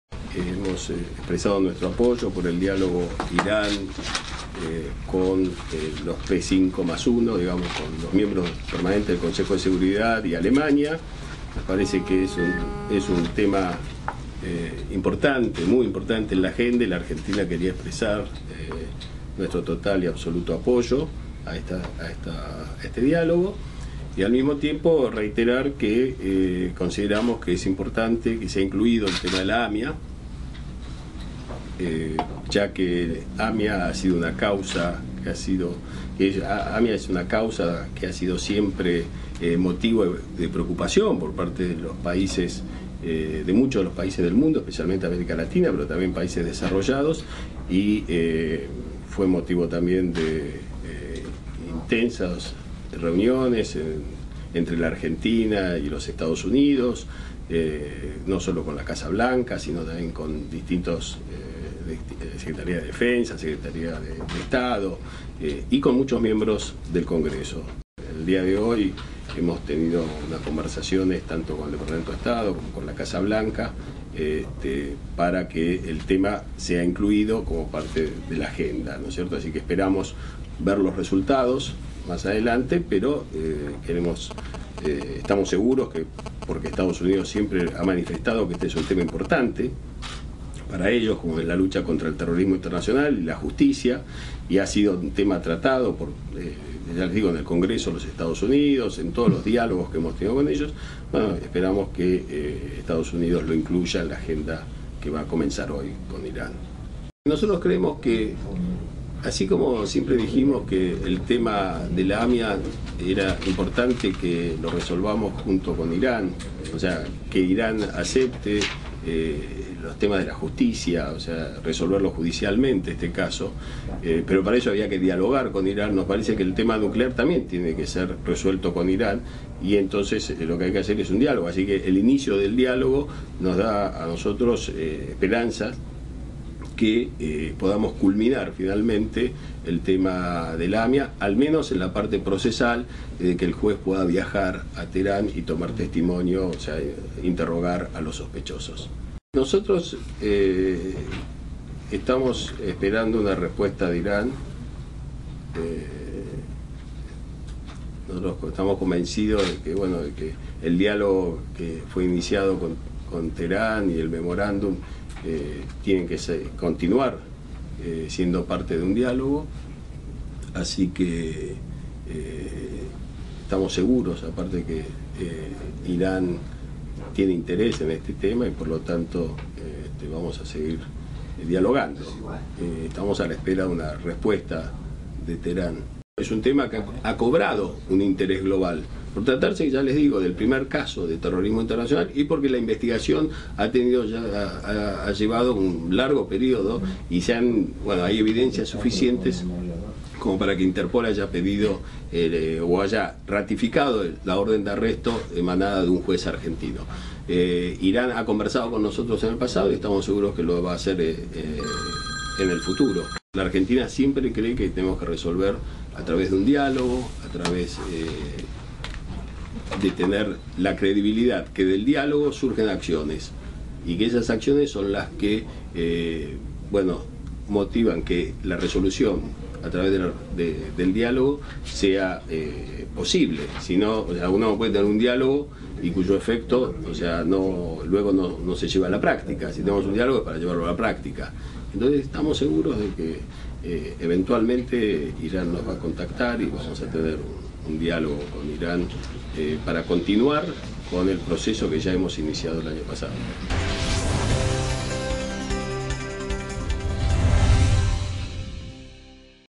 Timerman se reunirá con su par iraní en la ONU para hablar sobre el memorando
El gobierno de Irán solicitó un encuentro con el canciller Héctor Timerman para abordar la situación en que se encuentra el memorando de entendimiento firmado entre ambos países para avanzar en la investigación del atentado a la AMIA, el cual se concretará el sábado próximo en la sede de la ONU, y en el cual Argentina planteará, entre otros puntos, en qué fecha viajarán las autoridades judiciales argentina a Teherán.
Así lo afirmó esta tarde el propio Timerman, quien el sábado por la mañana mantendrá la reunión con su par Javad Zarif, a quien "se le plantearán los temas abordados por la presidenta Cristina
Fernández de Kirchner en su discurso" (del martes último ante la 68ª Asamblea de la ONU), precisó el canciller.
Puntualmente, Argentina intentará saber si fue aprobado el memorando por el Congreso de Irán, y en caso positivo conocer cuándo quedaría conformada la Comisión de la Verdad y, por último,
acordar el viaje de representantes de la justicia Argentina a Teherán.
La Comisión de la Verdad está previsto que sea integrada por juristas internacionales para asesorar a la investigación, y no para investigar, como informó erróneamente Télam en un despacho anterior.
"Hemos recibido un llamado de parte de las autoridades de la República Islámica de Irán mediante el cual nos han solicitado tener una reunión, en el marco de las Naciones Unidas, el día
sábado entre los dos cancilleres para tratar el tema del memorando", explicó Timerman a los medios presentes.
En ese sentido, dijo que el gobierno argentino "aceptó el diálogo, como siempre" y que planteará "conocer el estado actual del memorando en la legislación iraní, es decir, saber si fue aprobado por el Congreso o no, y si fue aprobado, establecer el marco con el cual vamos a formalizar la fecha de conformación de la comisión y el viaje de autoridades judiciales a Teherán".
El jefe de la diplomacia argentina recordó que "la Comisión no es vinculante" y que la Justicia "tiene su propia dinámica, es otro camino".
Además remarcó que el requerimiento del contacto de parte de los funcionarios iraníes "fue puntualmente por el tema del memorando" y fue hecho por "el canal diplomático".
"Argentina ratifica su pedido realizado a los Estados Unidos -a la Casa Blanca directamente- para que el tema de la causa AMIA se trate en el diálogo que inician hoy el P5+1, compuesto por los 5
miembros permanentes del Consejo de Seguridad más Alemania", añadió.
Timerman señaló que "ese pedido se produjo hoy" y que se trata "de un tema importante que hace a la agenda global".
"Argentina pretende que este tema sea tocado en las reuniones del P5+1 con Irán", concluyó desde Nueva York.